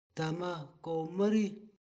tamah ko mari ("tsamma pap" in Ju|'hoansi)
Ju|'hoan audio clip for plant posters.

tamah ko mari (Ju|'hoansi)

tsamma pap (English)